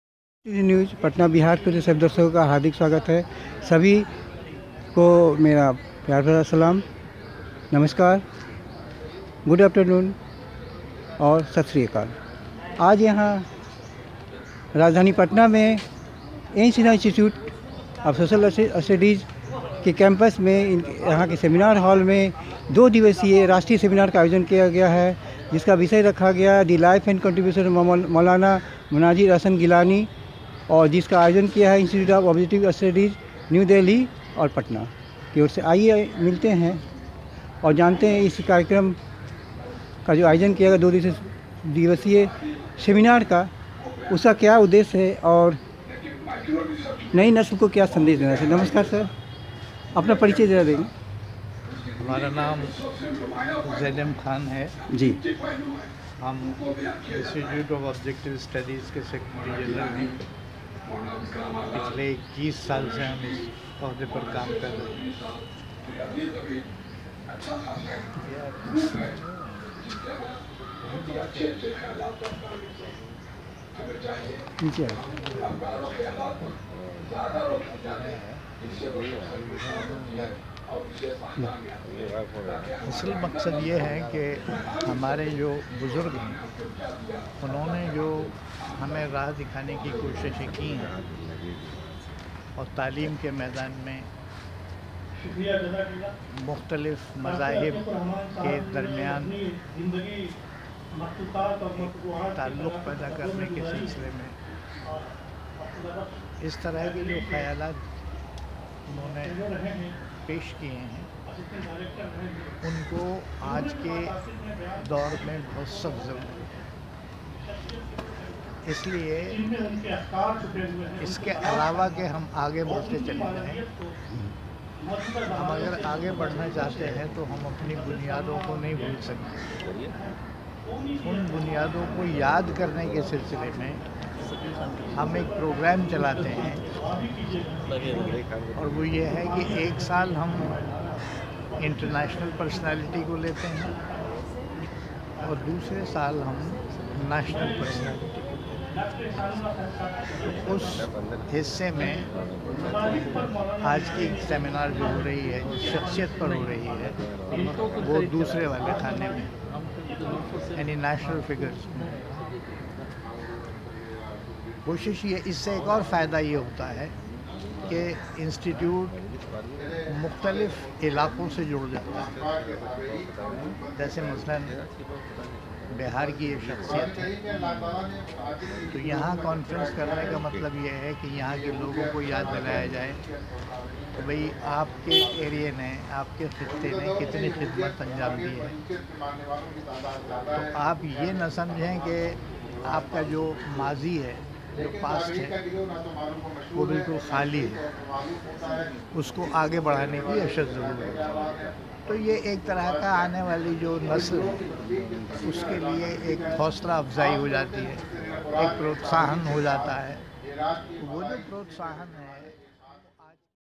Always Remember the Contributions of Seniors for Better Future of Youths : Prof. Z.M. Khan
| Amritnidhi News | SUBSCRIBE, LIKE, COMMENT & SHARE | Education | Institute of Objective Studies, New Delhi | Prof. Z.M. Khan, Genius Secretary General, IOS, New Delhi | Two Day National Conference on The Life and Contributions of Maulan Manazir Ahsan Gilani, 1 & 2 December 2018 ANSISS, Patna | Video Journalist : Ranjan Kumar Amritnidhi | E-mail : | THANKS FOR WATCHING